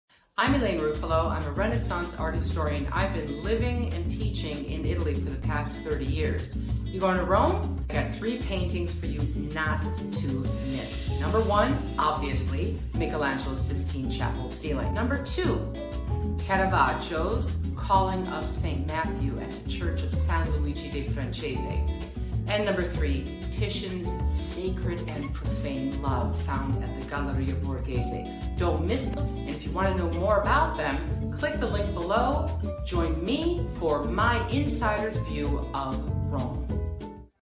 A Traveler in Italy: Rome: Top 3 Paintings NOT to Miss!
Wondering what art you just cannot miss while in Rome?  Here are my top 3!

The obvious heavy hitter: Michelangelo, Sistine Chapel Ceiling
A chapel in the Apostolic Palace, the official residence of the pope, in Vatican City.

The Calling of St Matthew: Caravaggio
Housed in the Contarelli Chapel in the church of the French congregation, San Luigi dei Francesi.

Sacred and Profane Love by Titian at the Galleria Borghese